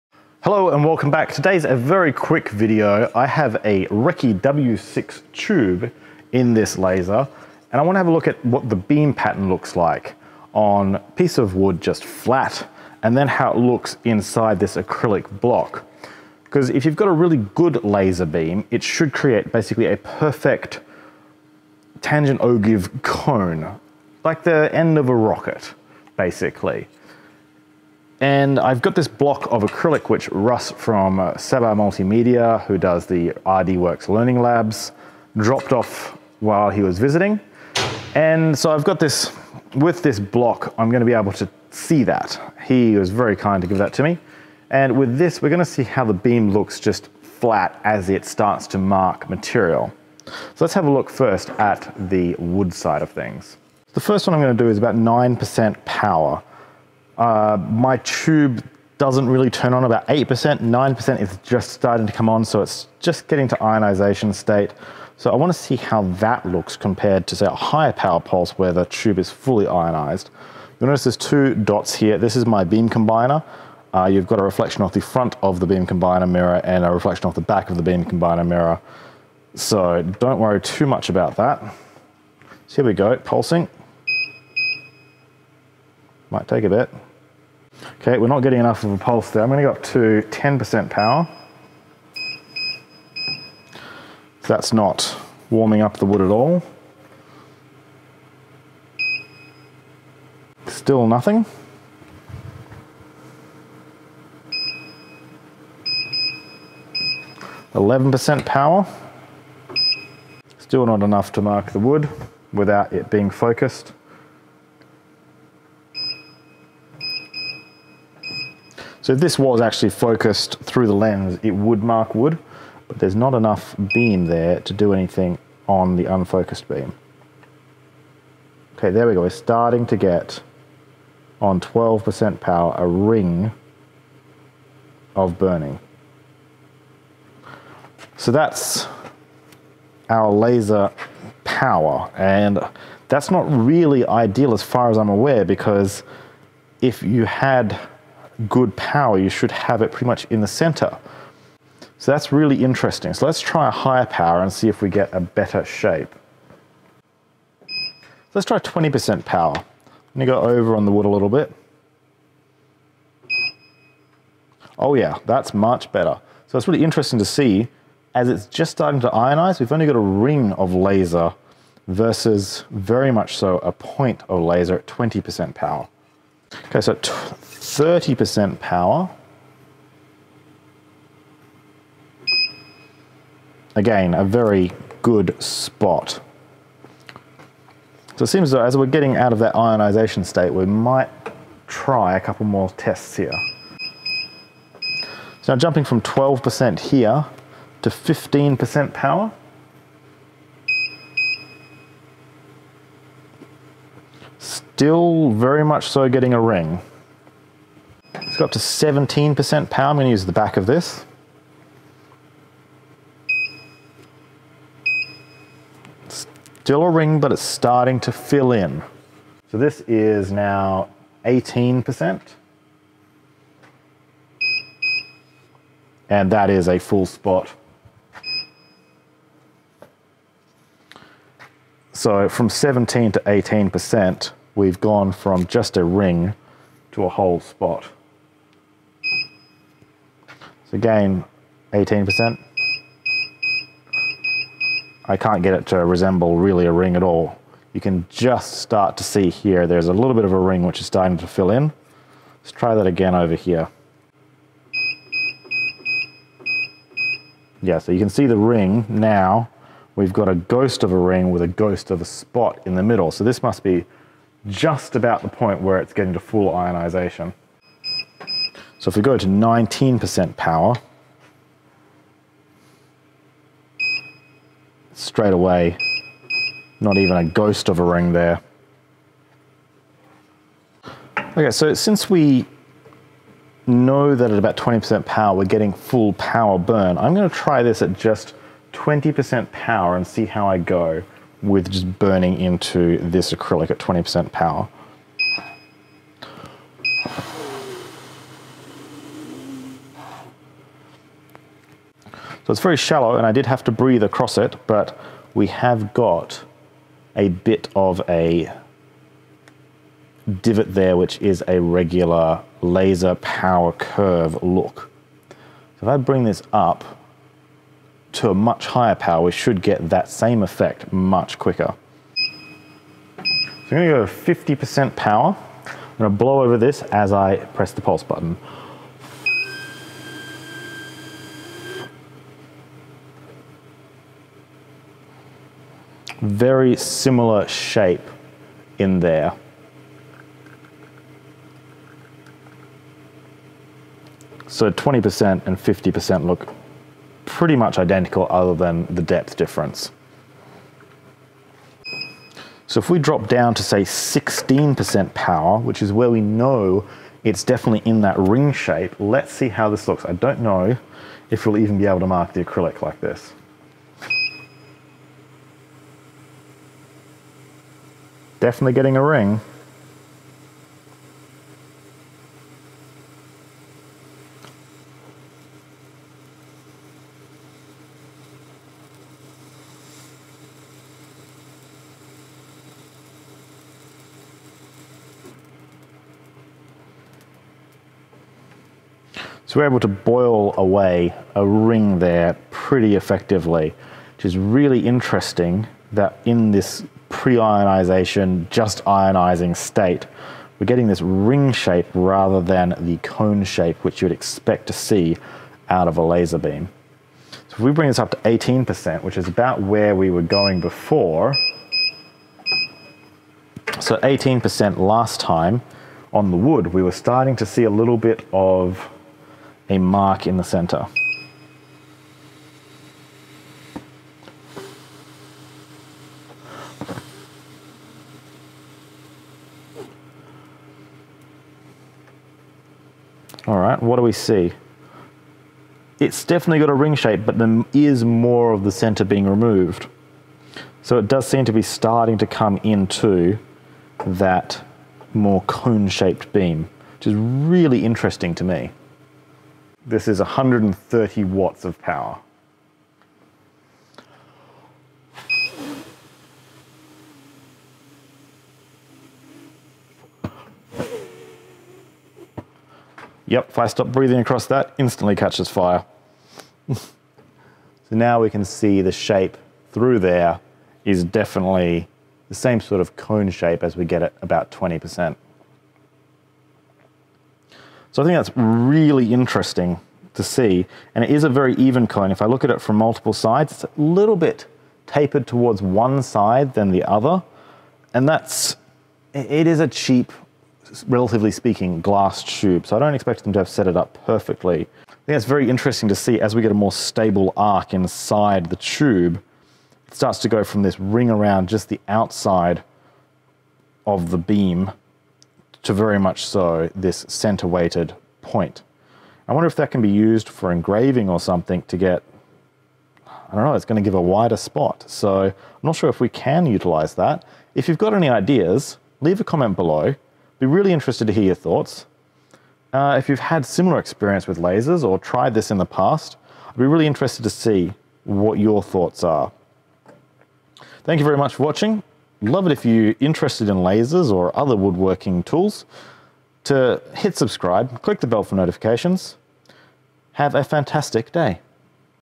Laser Beam Shape Testing - The Woodshop Life
This test was done with a RECI W6 tube on a RECI P18 powersupply.